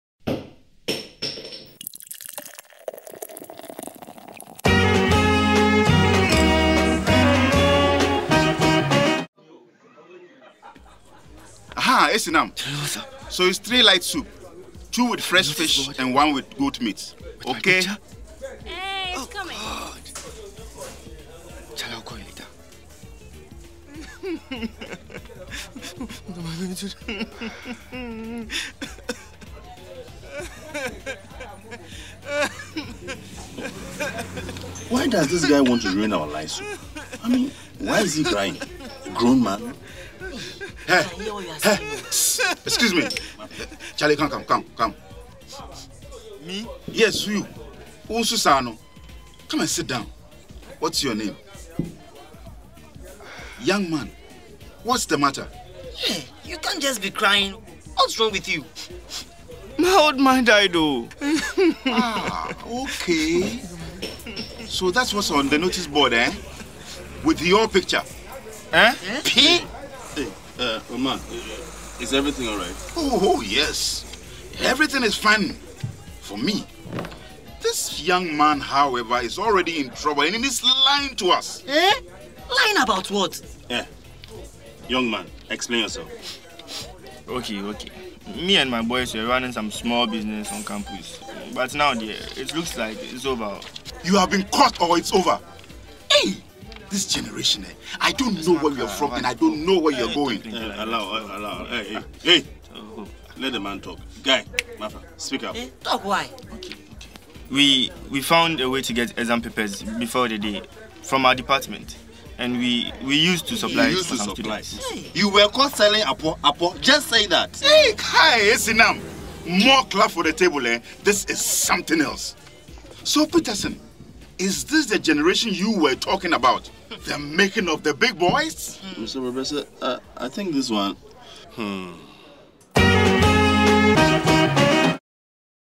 CLUB BEER AT THE BAR EPISODE 2
Charlie the men ‪#‎AtTheBar‬ are trying to figure out why today's generation behave the way they do. What's your take on this topic?